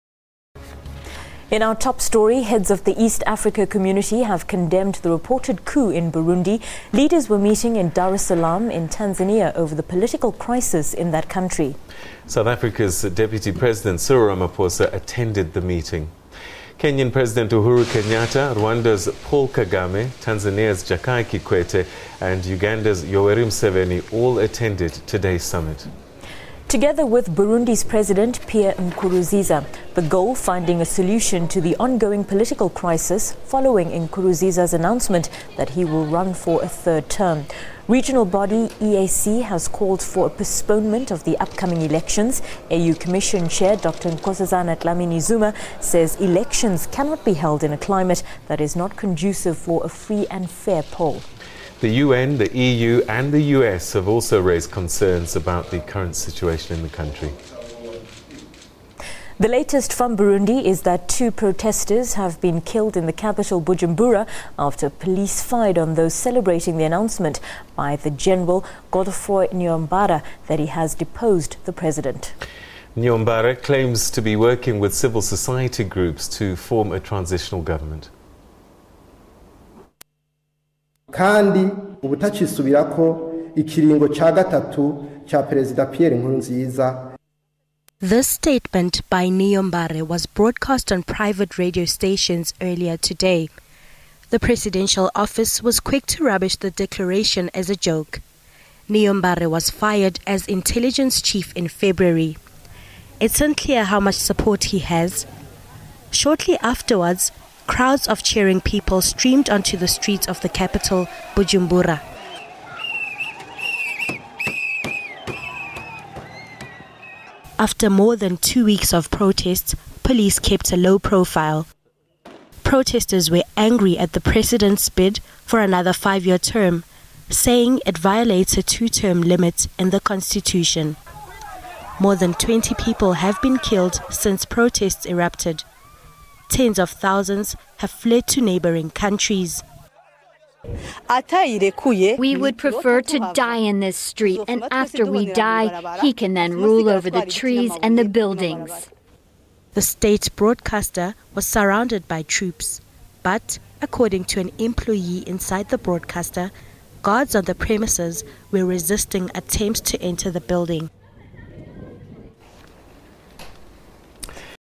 Burundians celebrate the dismissal of President Nkurunziza
The latest from Burundi is that two protestors have been killed in the capital Bujumbura after police fired on those celebrating the announcement by General Godefroid Niyombare that he has de-posed the president. General Niyombare claims to be working with civil society groups to form a transitional government.